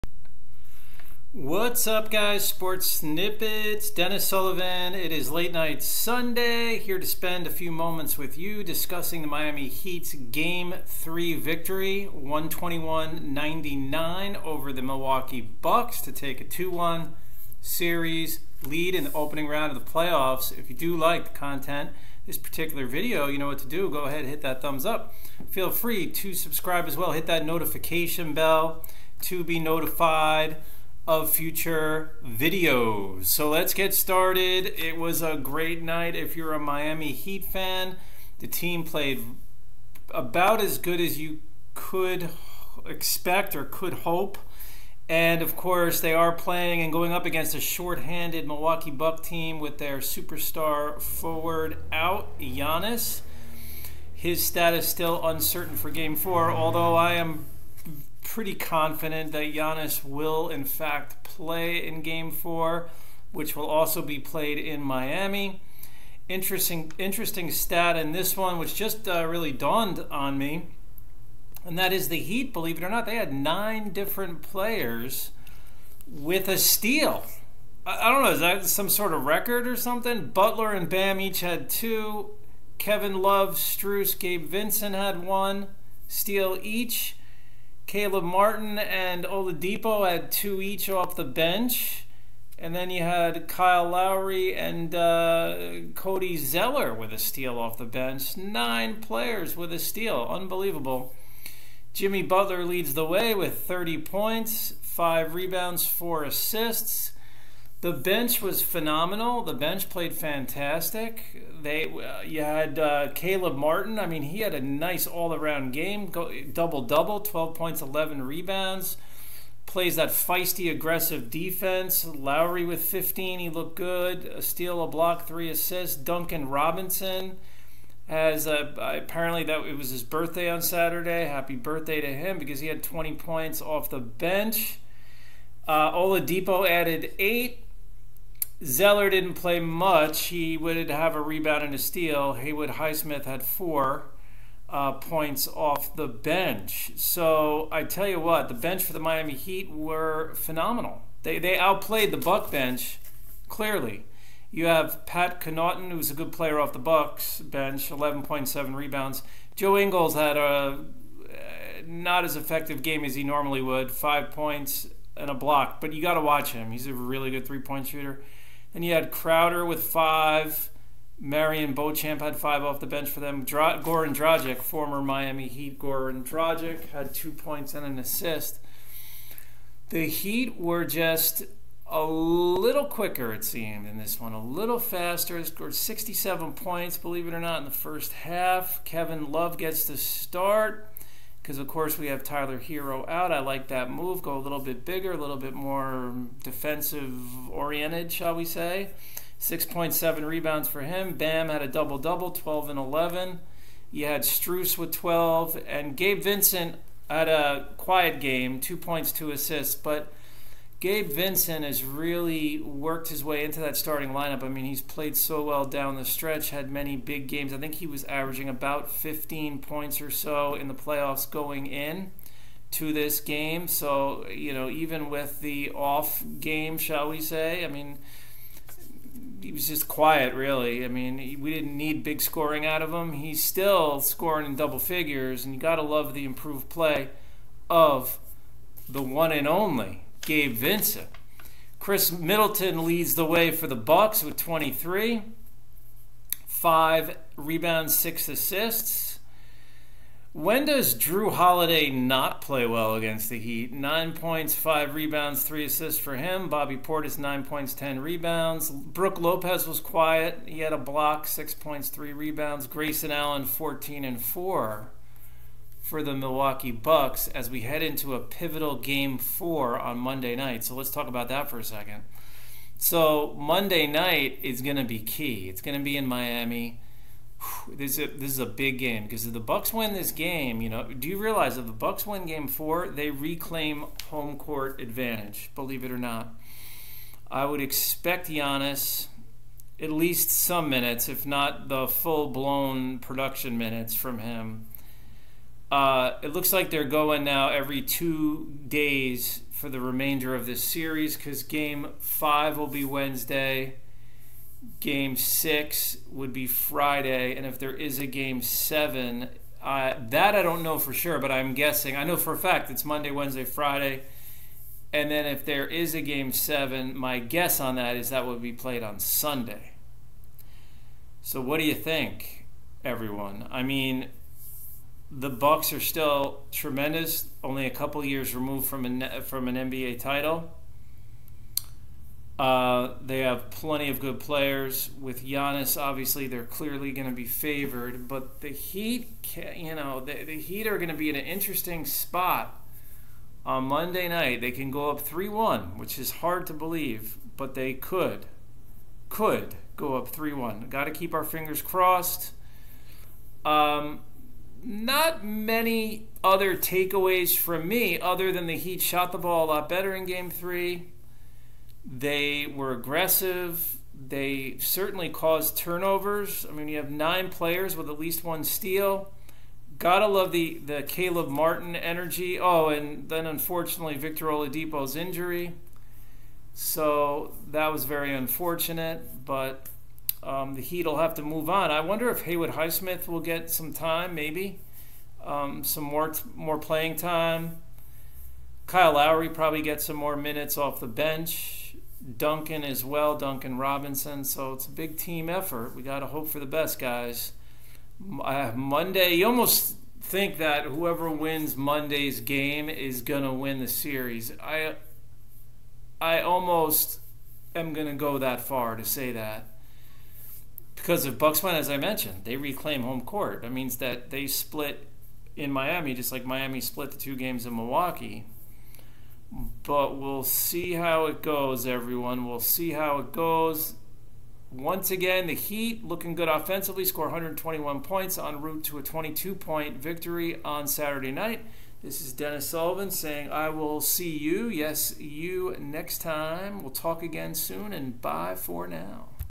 Miami Heat take Game #3 vs Milwaukee Bucks 121-99....Game #4 will be PIVOTAL!
Jimmy Butler scored 30 points, as the Miami Heat would defeat the Milwaukee Bucks by a score of 121-99 on Saturday Night to take a 2-1 series lead in the opening round of the playoffs.  Khris Middleton would lead the Bucks with 23 points, as Milwaukee looks for the return of their star forward Giannis for Game #4 Monday Night...